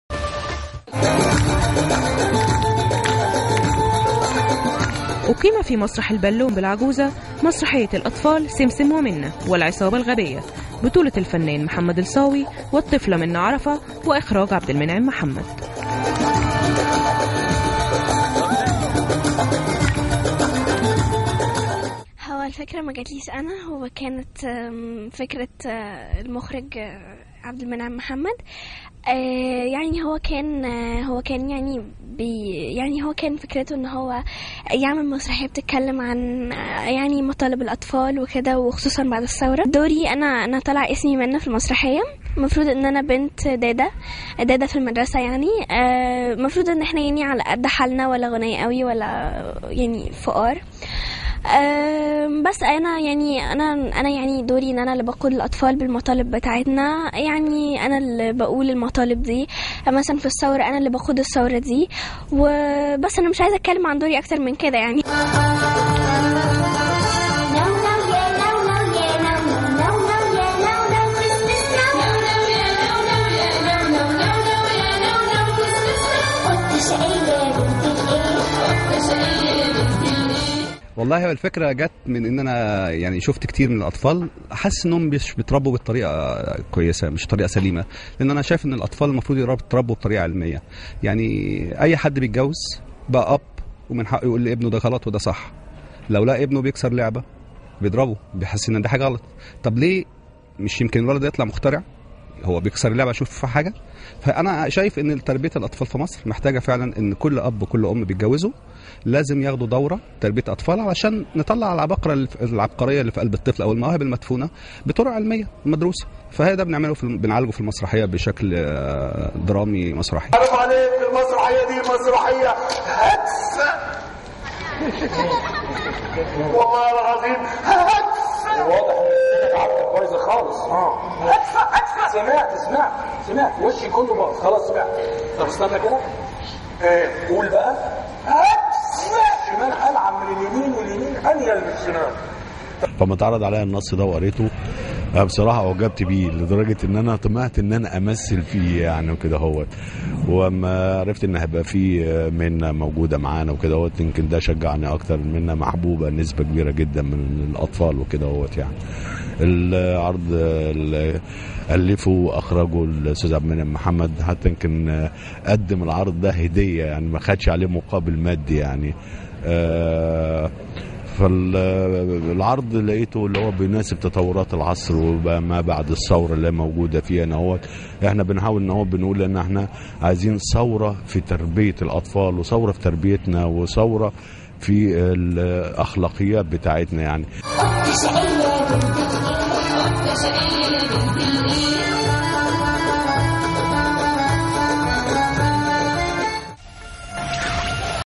تقرير حول مسرحية الأطفال سمسم و منة و العصابة الغبية
تقرير حول مسرحية الأطفال سمسم و منة و العصابة الغبية التى أقيمت بمسرح البالون بالعجوزة
حلقة السبت 8 أكتوبر 2011